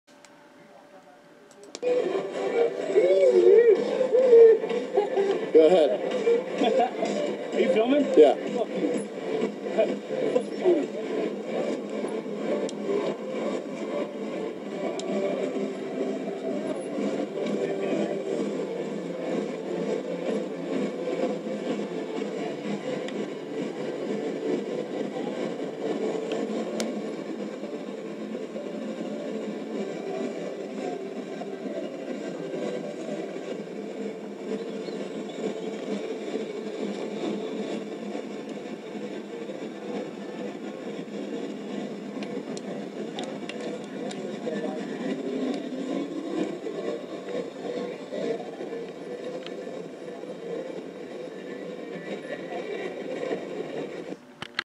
Couple of Copwatcher's walking by
they recorded a traffic stop of a guy on a bike